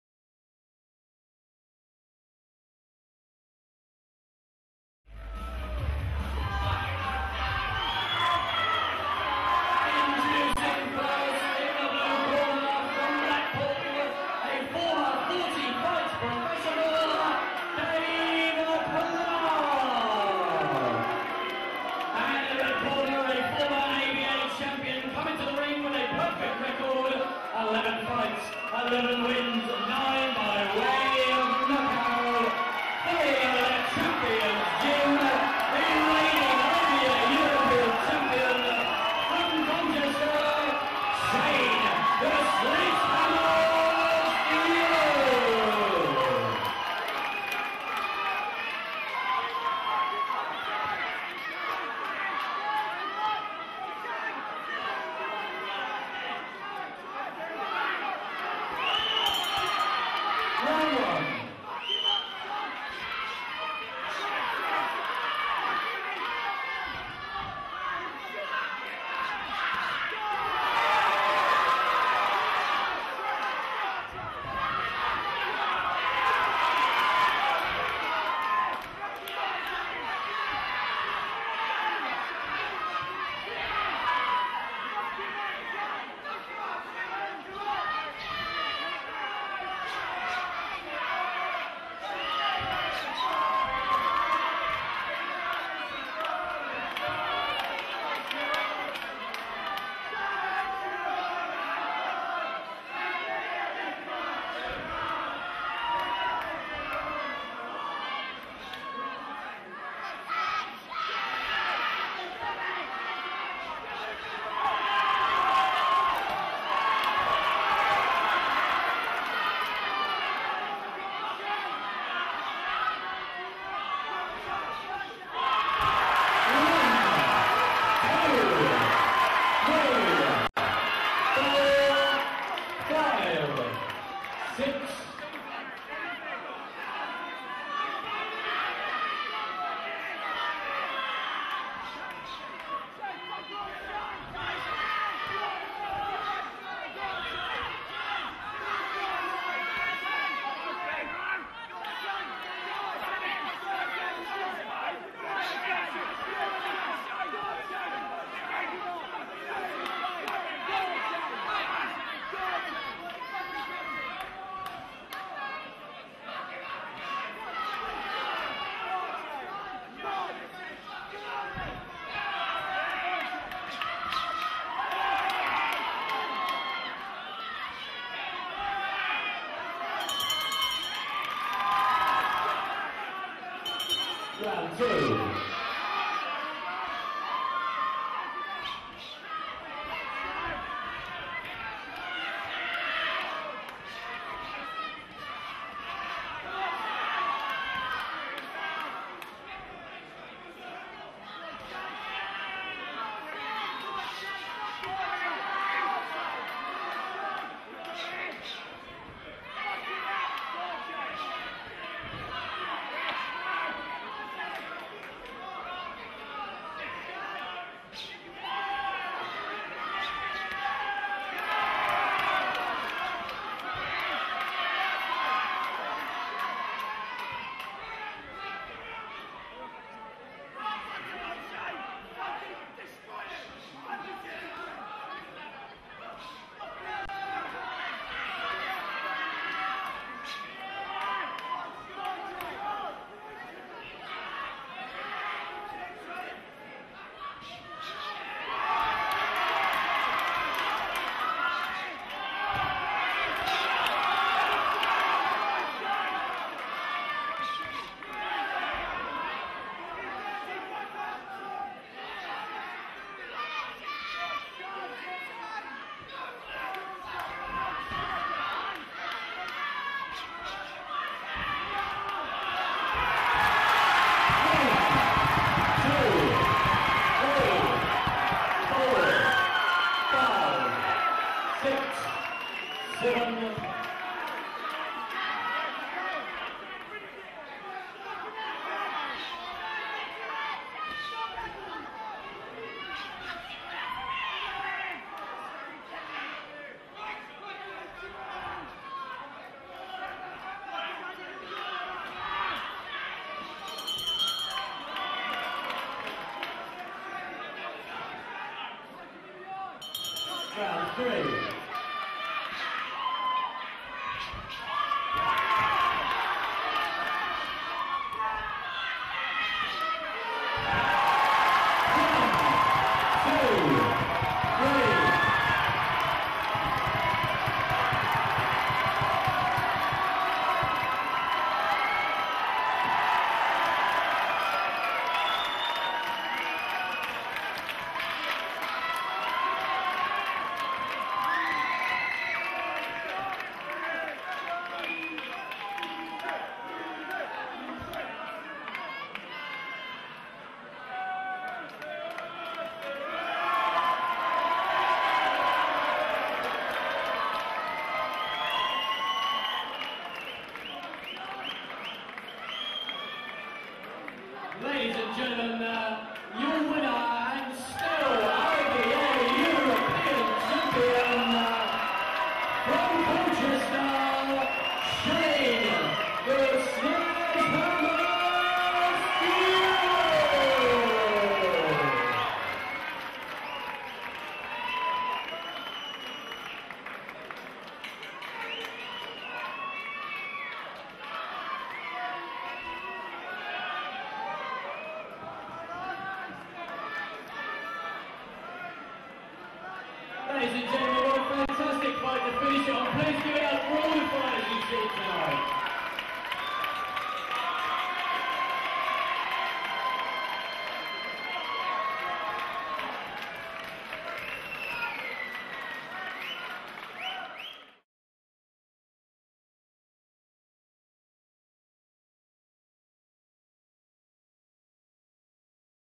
IBA Essex Boxing - Shane Steward v Dave Clarke - Liquid Colchester
IBA Essex Boxing fight between Shane Steward & Dave Clarke at The Liquid Nightclub Colchester on April 25th 2010. Footage supplied by Liam Galvin & Yvette Rowland